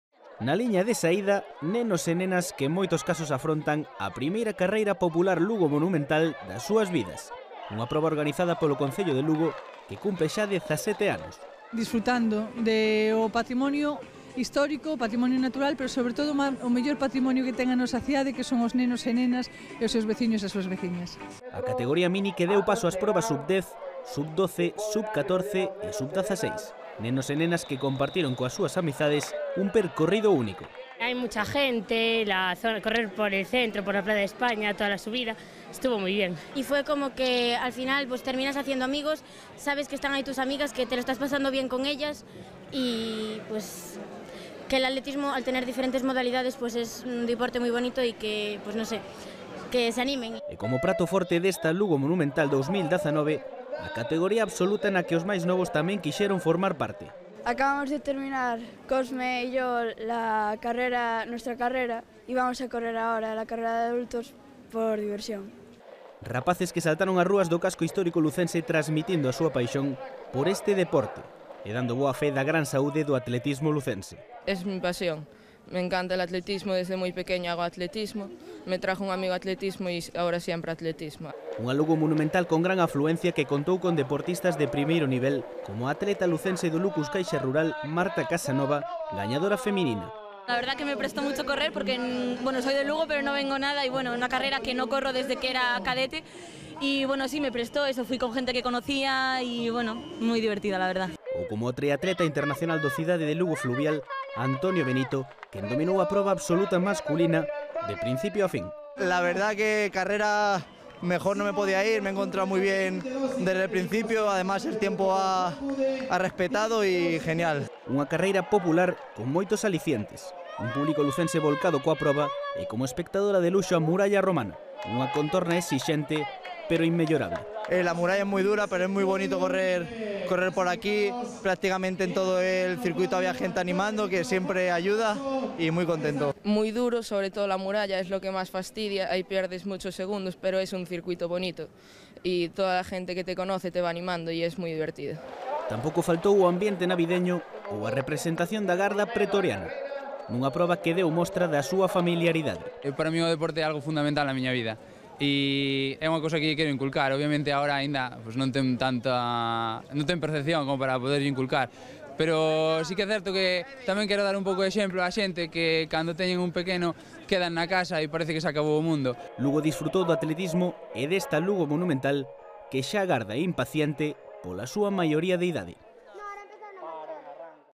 A Muralla volve ser testemuña da Lugo Monumental
Na liña de saída, nenos e nenas que, en moitos casos, afrontan a primeira carreira popular Lugo Monumental nas súas vidas. Unha proba organizada polo Concello de Lugo que cumpre xa dezasete anos.